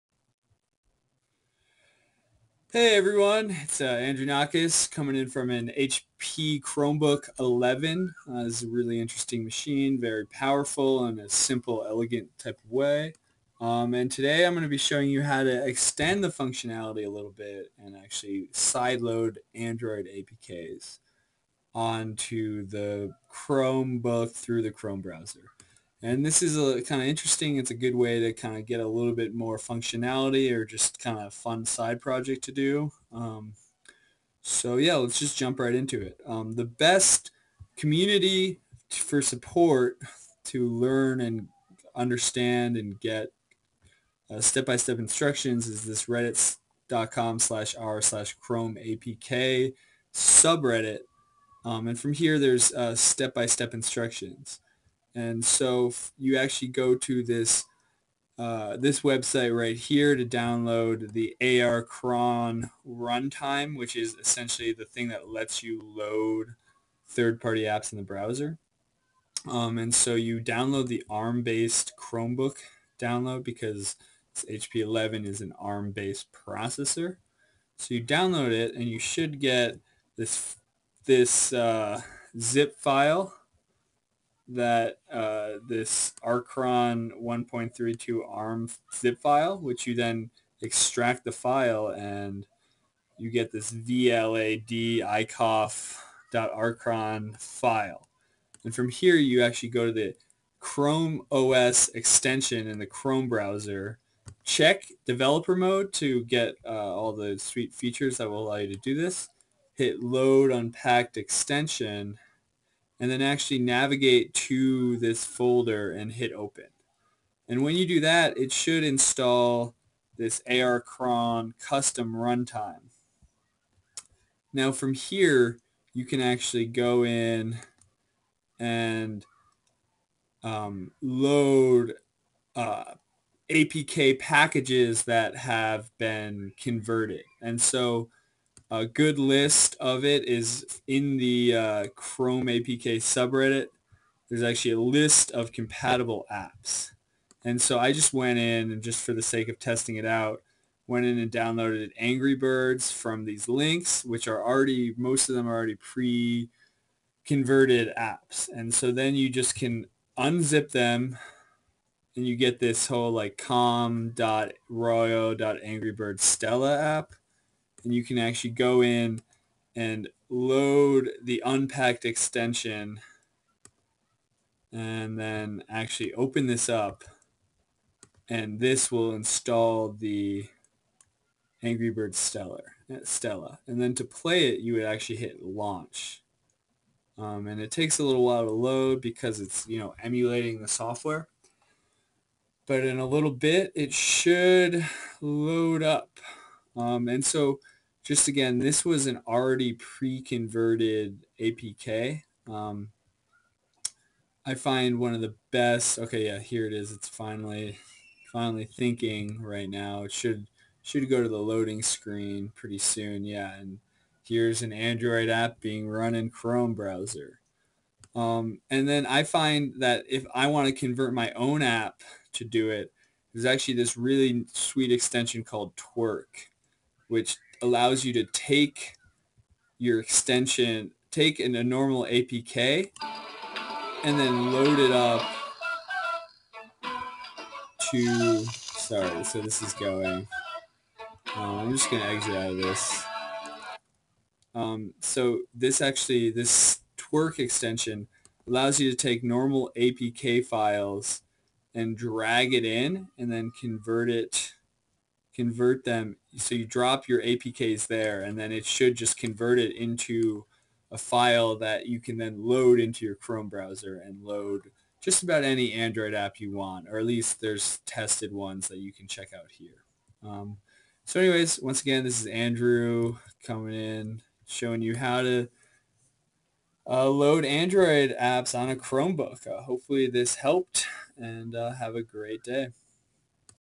How to Side Load Android APKs on Chromebooks using Archron
This is a how to screen cast going over the steps needed to side load Android Apps onto a ChromeBook.